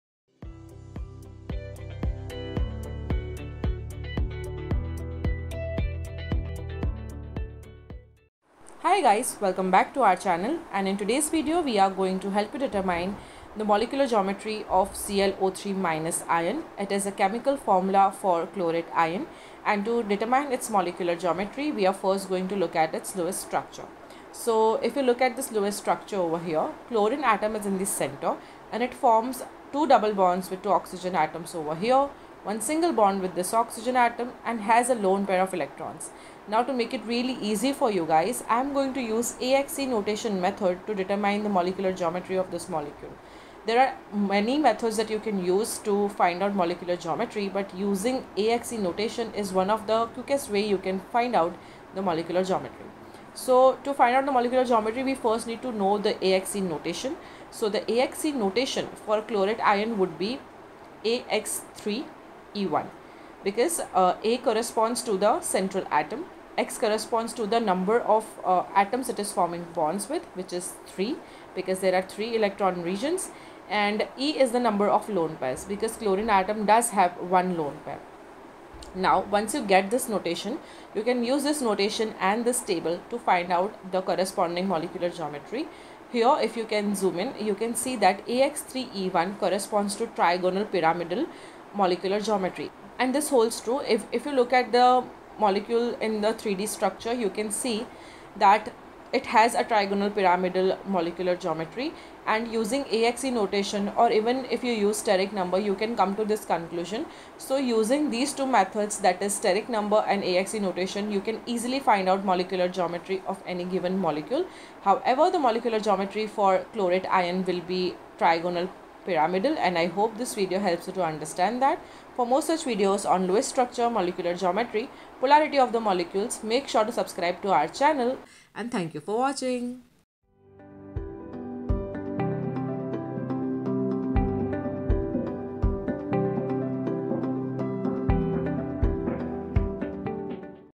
ClO3- Molecular Geometry, Shape & Bond Angles (Chlorate Ion)
Hello Guys!

Chlorate ion or ClO3- ion comprises one Chlorine atom that is in the center forming bonds with three Oxygen atoms. To find out its Molecular Geometry we first examine its Lewis structure to know the arrangement of electron pairs and find out its AXE notation.

This notation helps in finding out the molecular geometry of this ion. Watch this video to find out Chlorate ion’s molecular geometry.